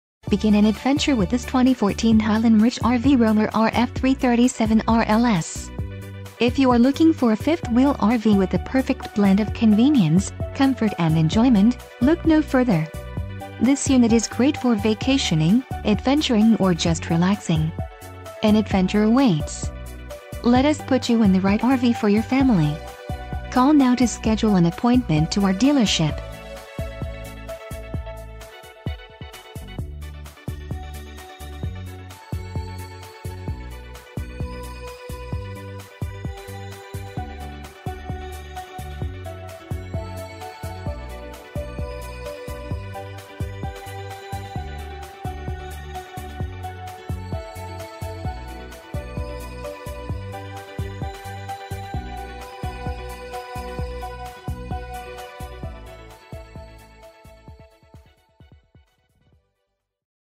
2014 Highland Ridge RV Roamer RF337RLS - Consignment RV ...
For Sale in Carthage, MO 64836

Description: 3 Slides Rear Living, Front Queen, 2 A/C's. All Aluminum Construction, 1/2 in. Plywood floor and roof decking Four season Insulation, PVC Roof NOT RUBBER!Features:Gross Vehicle Wt Rating (GVWR): 12,340 lbs.Gross Axle Wt Rating (GAWR): 10,400 lbs.Unloaded Vehicle Wt (UVW): 7,465Cargo Carrying Capacity (CCC): Gross: 3,500 lbs.Axle Weight: 6,900 lbs.Hitch Weight: 1,940 lbs.Length Feet: 33.5Overall Length: 33 ft. 6 in.Exterior Height: 155 in. with A/C on roofExterior Width: 100 in.Wheel/Tire Size: 16 in.No. of Slide-Outs: 3Exterior Storage: YesAwning Length: 16 ft.Fresh Water Capacity: 85 gal.Gray Water Capacity: 57 gal.Black Water Capacity: 41 gal.Furnace BTU Rating: 35,000Water Heater Capacity: 10 gal.Propane Tank Capacity: 80 lbs.Standard Package (Required): Four Seasons Protection Package, Executive Package